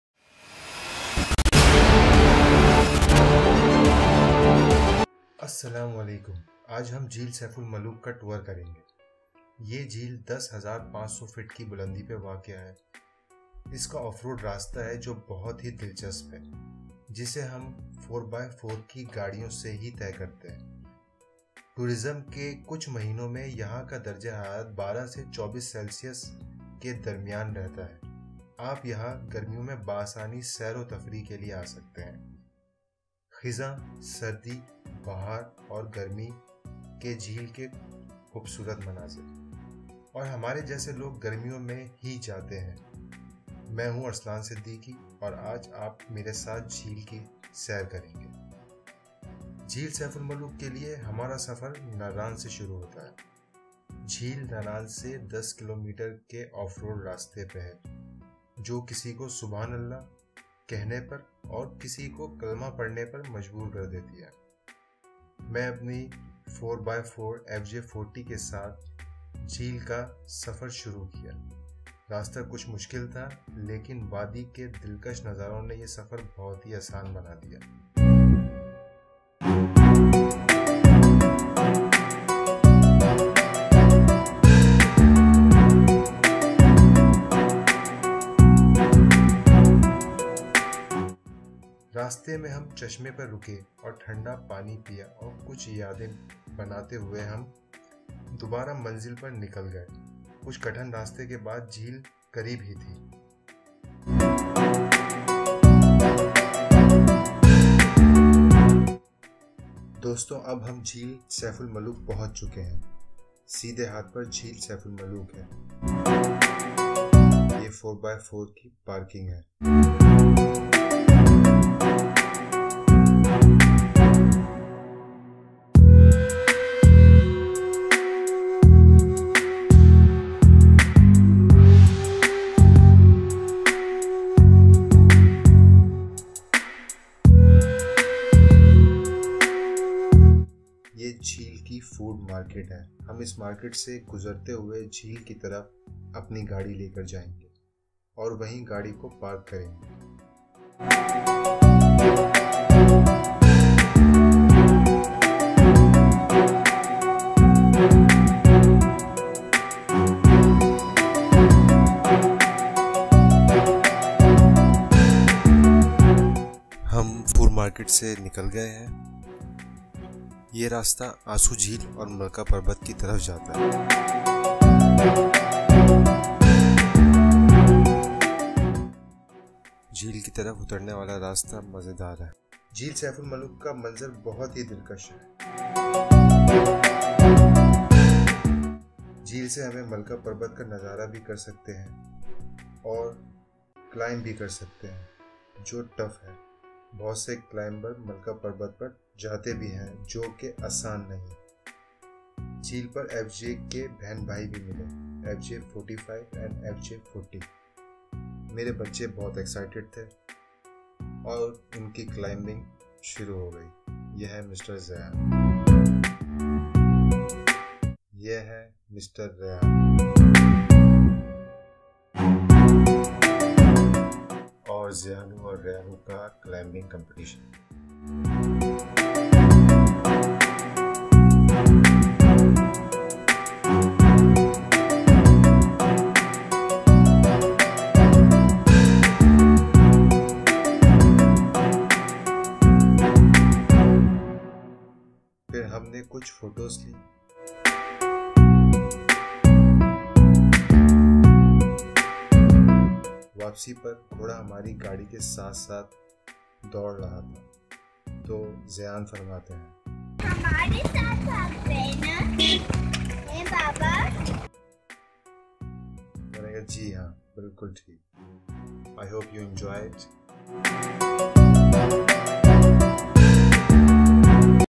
Saif Ul Malook Jheel (Lake) Naran, Pakistan, DISCOVERISTAN, in 5 minutes
This is the short trip story of Saif ul Malook lake with my family which is beauteous valley and Lake of Naran, Pakistan.I displayed four seasons of Saif Ul Malook Lake. I Showed the way of Malika Parbat and the Ansu Jheel(Lake). You can go with your 4x4 offroad to nearest point of this lake. You can go there in summer only with your 4x4 and can go with the local drivers 4x4 Jeeps. Don't go with your car.